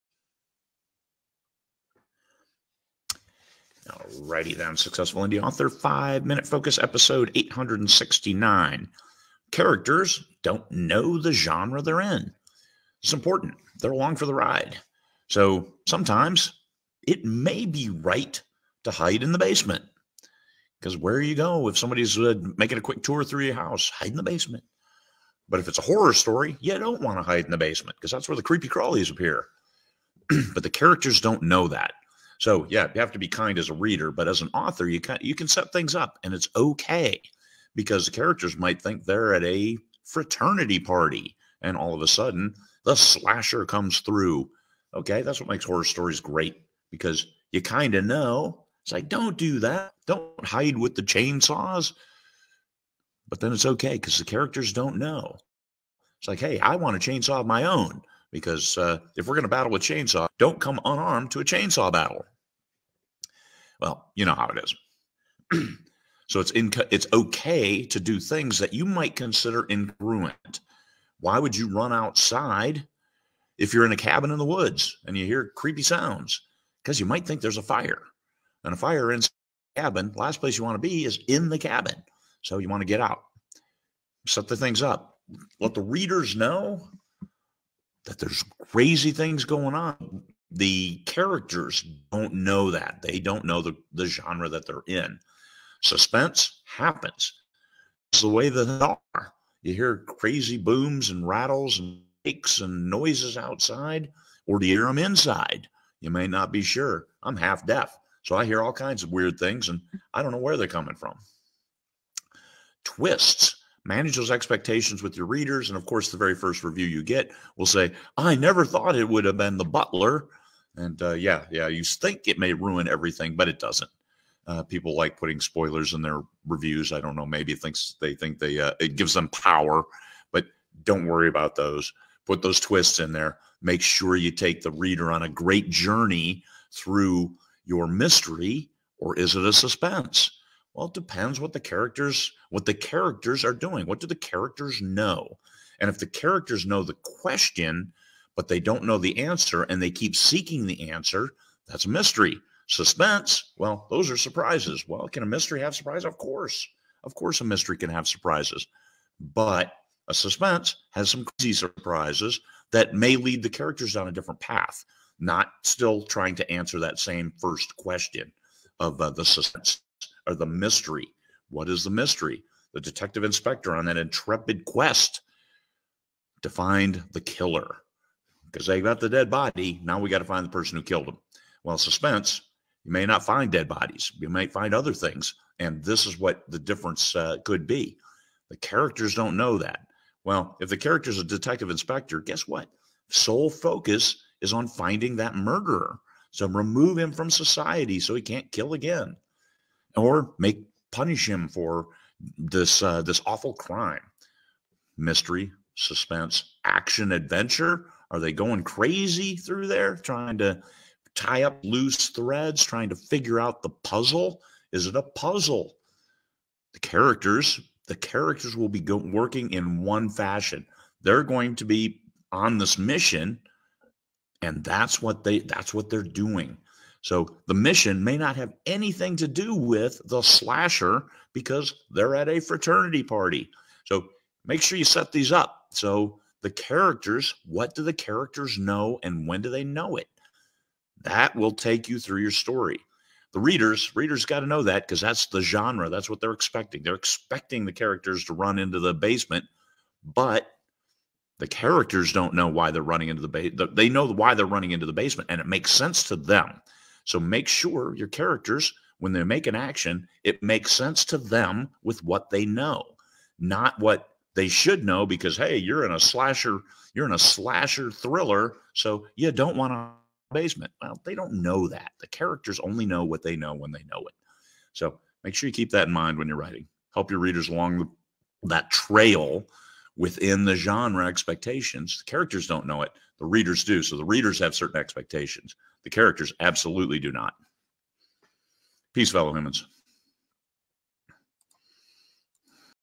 Successful Indie Author Five Minute Focus Ep869 - Characters Don’t Know the Genre of Their Stories
Characters don’t know the genre of their stories
1. They’re along for the ride, unlike the readers who have foreknowledge (we put the books in categories)
2. It’s okay if they do things incongruent with what you know – why not hide in the basement?
3. Suspense happens
4. Twists – manage expectations (subtle, not through brute force)
5. A subtle difference between mystery and suspense – what do the characters know?
6. 20Books Vegas 2023 Day 2 – Advanced Character Development